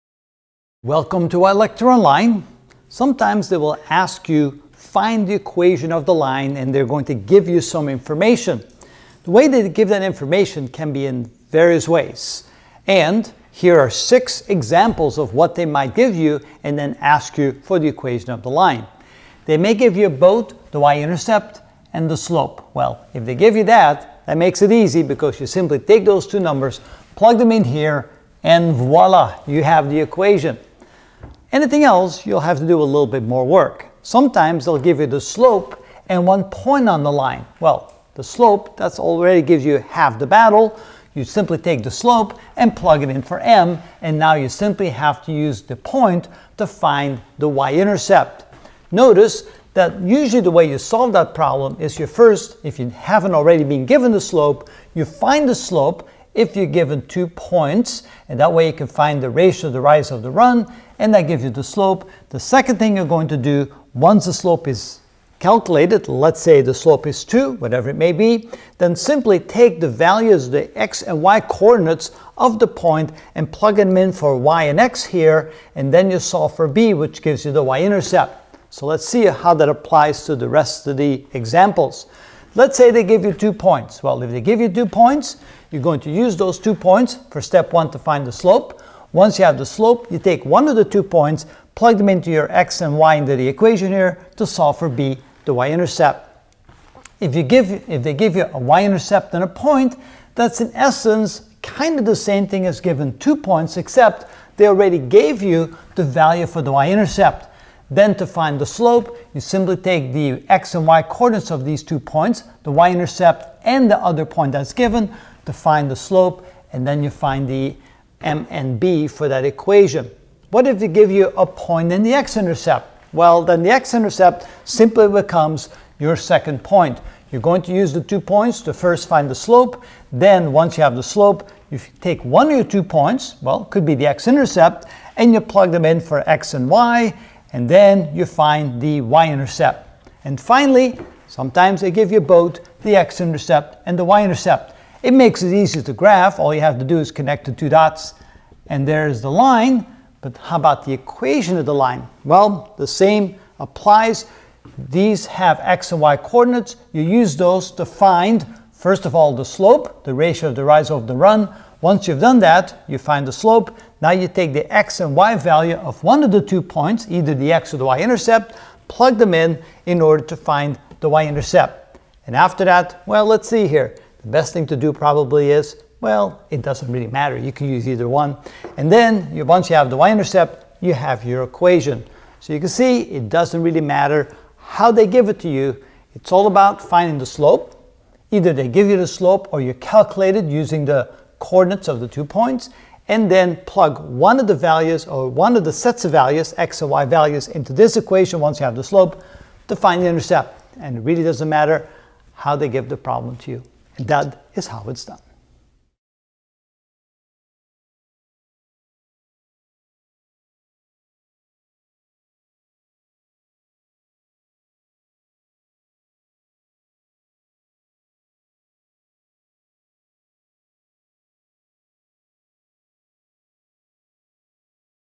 Algebra - Ch. 30: Linear Equations (14 of 33) How to Find the Equation of a Line? Given...
We will learn the six different ways to find the equation of the line given:
1) y-intercept and slope
2) slope and 1 point
3) 2 points
4) y-intercept and 1 point
5) 1 point and x-intercept
6) y-intercept and x-intercept